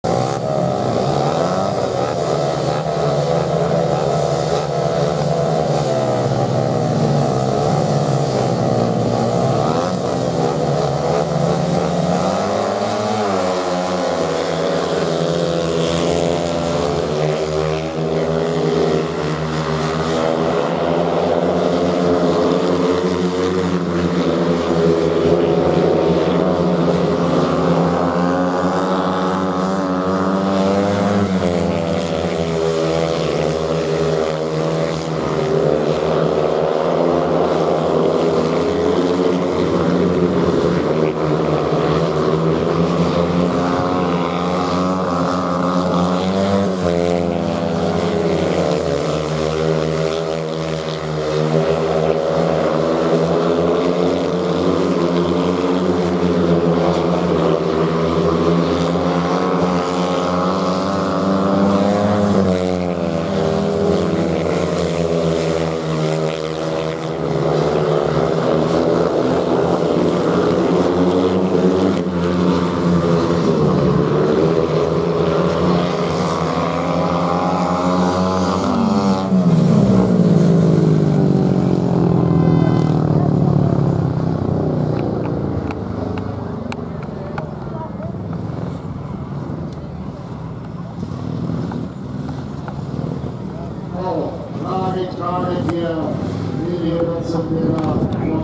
19.03.2016 - Ludwigslust
Int. Speedway - Heat 9: Geyer, Helmhout, Möser